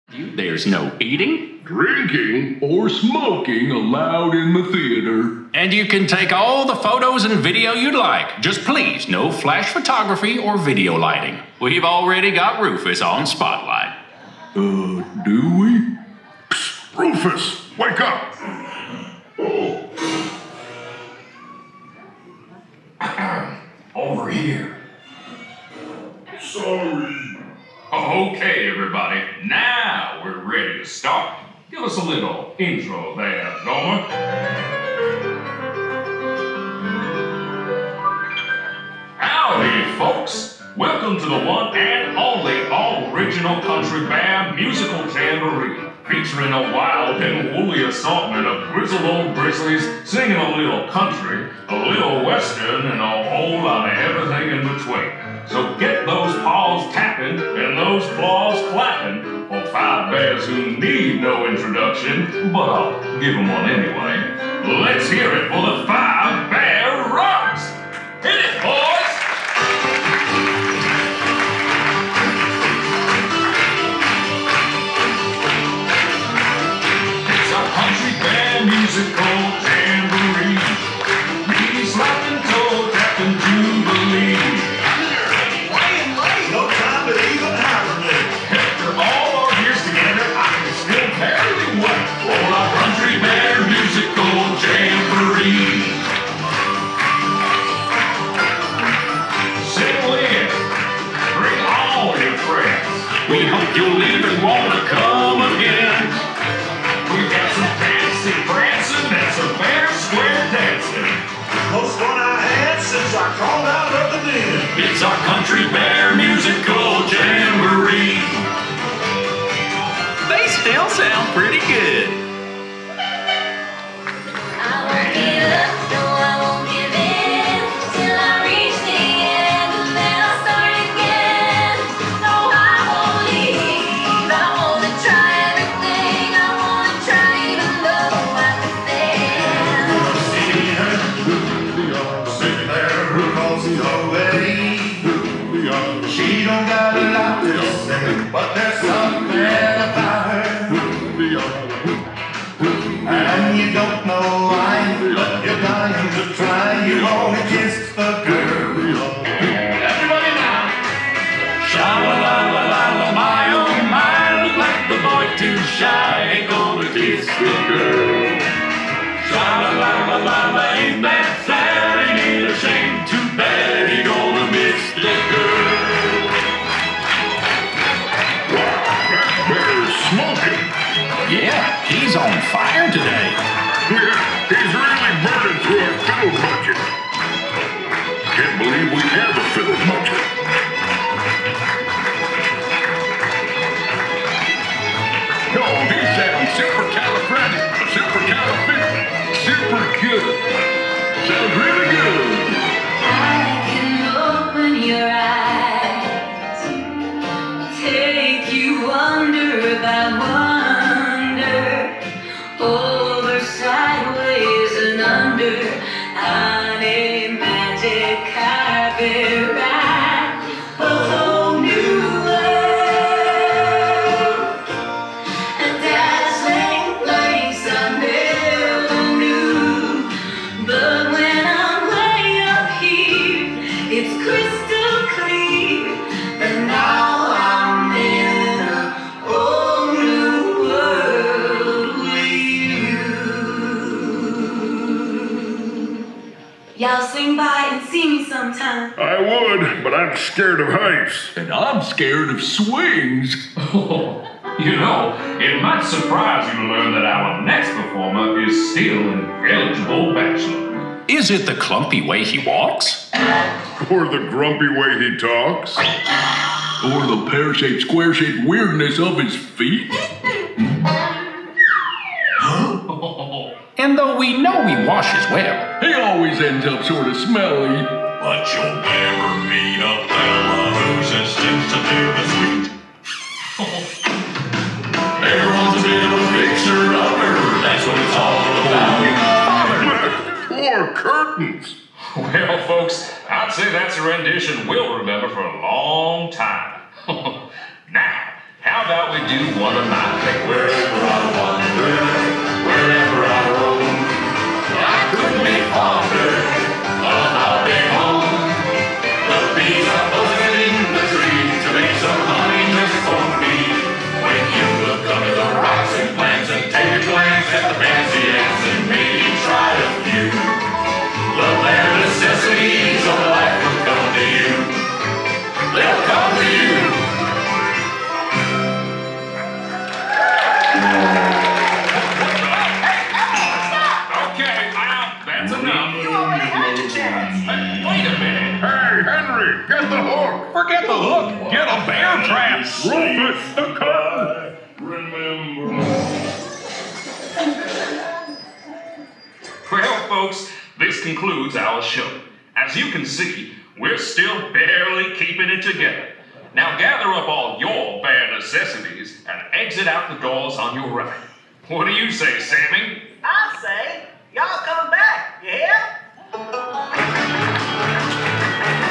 New Country Bear Musical Jamboree | Don't Panic, It's Actually Fine
Enjoy the NEW Country Bear Musical Jamboree show in this audience POV experience. The new songs and characters invoke a sense of nostalgic ambience with ear-worm enticing emotions that confuse and amuse the senses in a way that's almost un-BEARable.